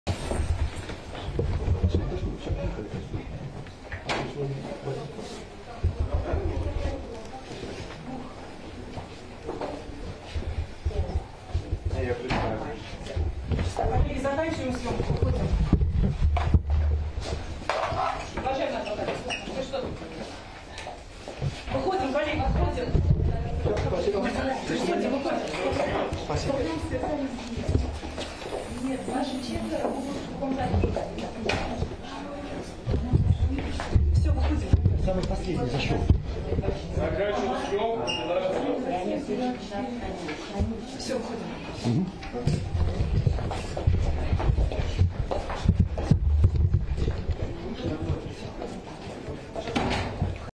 В суд МОСКВЫ ПРИВЕЛИ журналиста WSJ ЭВАНА ГЕРШКОВИЧА: первые КАДРЫ
18 апреля в Москве заключенного американского журналиста Эвана Гершковича доставили в суд, где состоится обжалование его задержания по обвинениям в шпионаже против РФ. За кадром слышно, как работники суда пытаются буквально "выставить" представителей СМИ за дверь.

Эван Гершкович – американский журналист WSJ, которого в марте 2023 года задержали в России по обвинению в шпионаже. В свою очередь, это стало первым случаем задержания журналиста США, начиная со времен Холодной войны.

Видео: AP